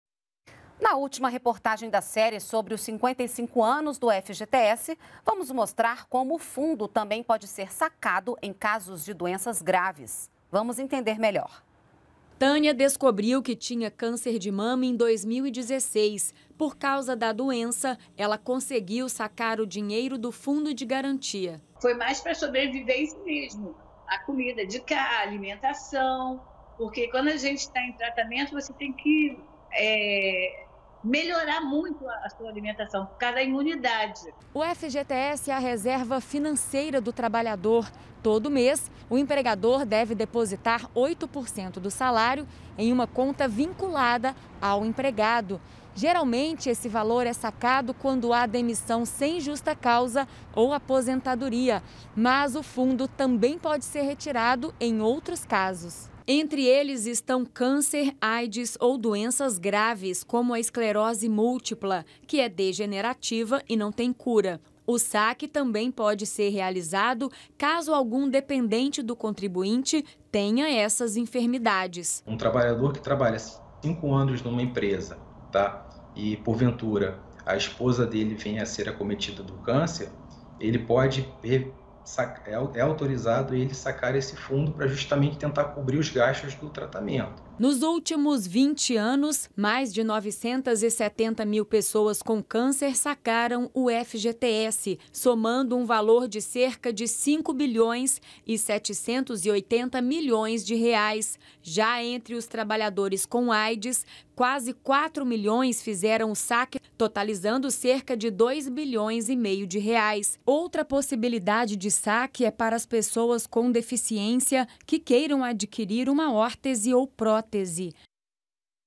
Saque do FGTS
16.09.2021 – Na última reportagem da série sobre os 55 anos do FGTS, informações sobre como o recurso pode ser sacado em casos de doenças graves.